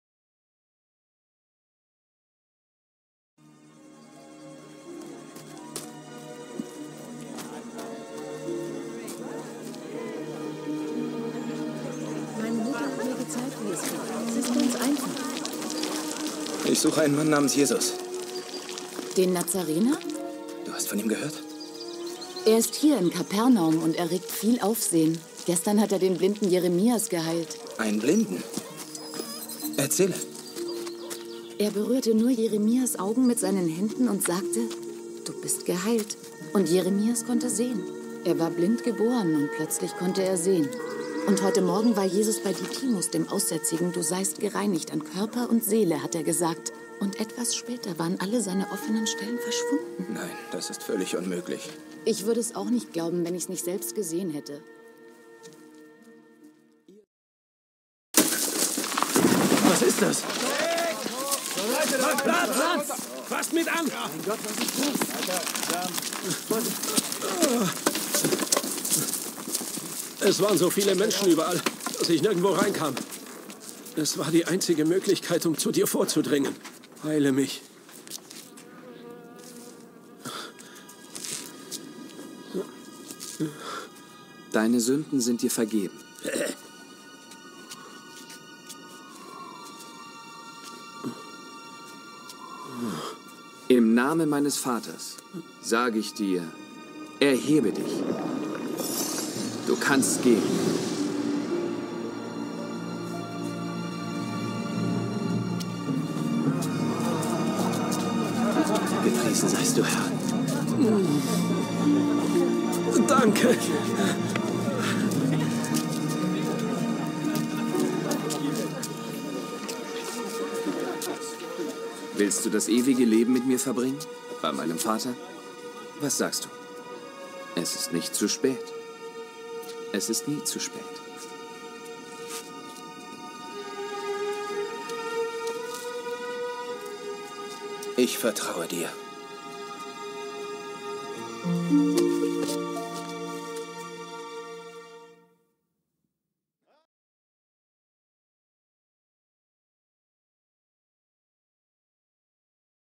DER MANN MIT NAMEN JESUS | Blinde sehen, Lahme gehen. Aussätzige werden geheilt
Bibelvideo